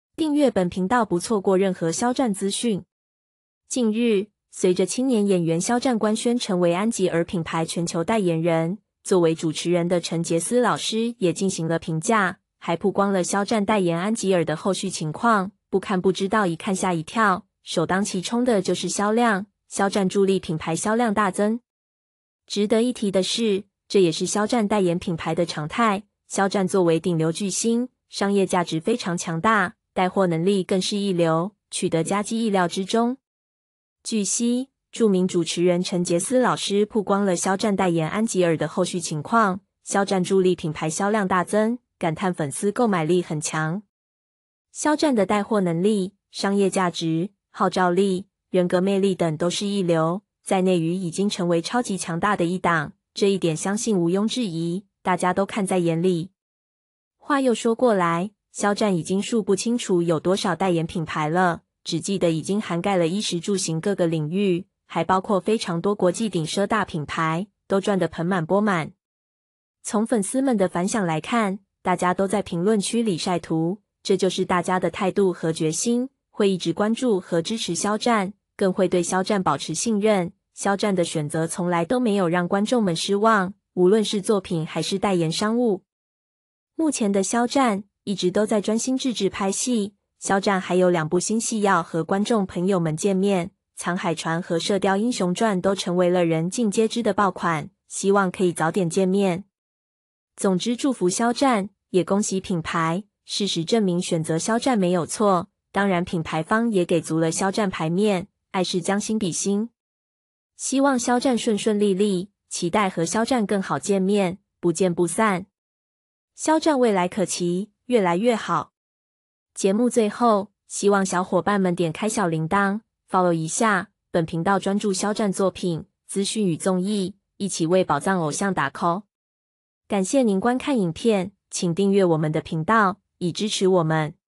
主持人曝光肖戰代言《安吉爾》後續！品牌銷量大增，帶貨能力一流
更新：频道主的个人感受：肖战真的有魅力在身上，我看见了你作为一个普通人，走向世界，绽放你的魅力，真的太励志了，太感动了，说句粗话，你真的有两把刷子在身上，我真的佩服你。